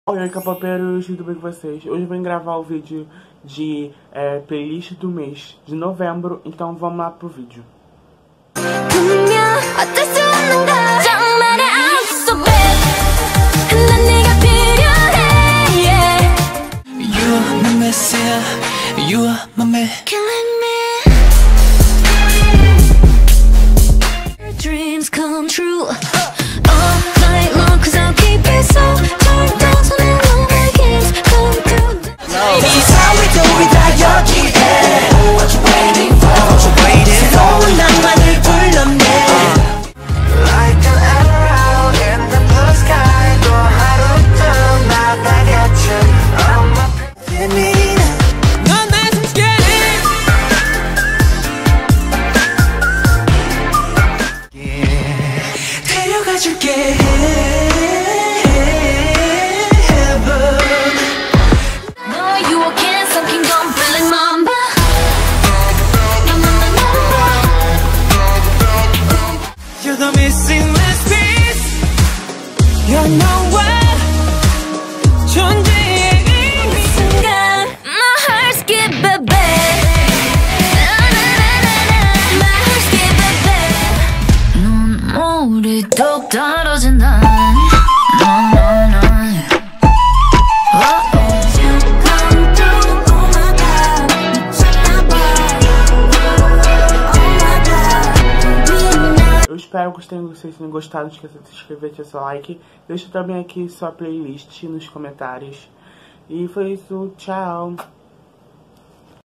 Playlist do mês| NOVEMBRO
Oi kapopeiros tudo bem? essa foi a minha playlist do mes de novembro, esses lançamentos foram meus favoritos. Qual foram os seus?